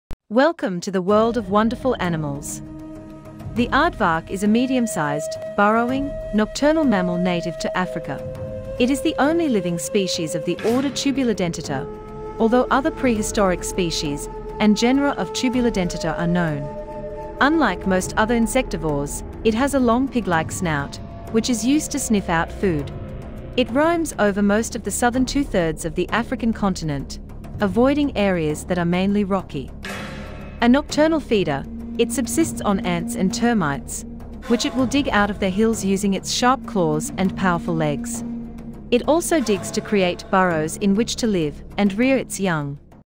Aardvark facts 🦌 🐜 termites eater 🥘 🤩
The aardvark is a medium-sized, burrowing, nocturnal mammal native to Africa.
It is the only living species of the order Tubulidentata, although other prehistoric species and genera of Tubulidentata are known.
Unlike most other insectivores, it has a long pig-like snout, which is used to sniff out food.
It roams over most of the southern two-thirds of the African continent, avoiding areas that are mainly rocky.
A nocturnal feeder, it subsists on ants and termites, which it will dig out of their hills using its sharp claws and powerful legs.
It also digs to create burrows in which to live and rear its young.

facts 101 for kids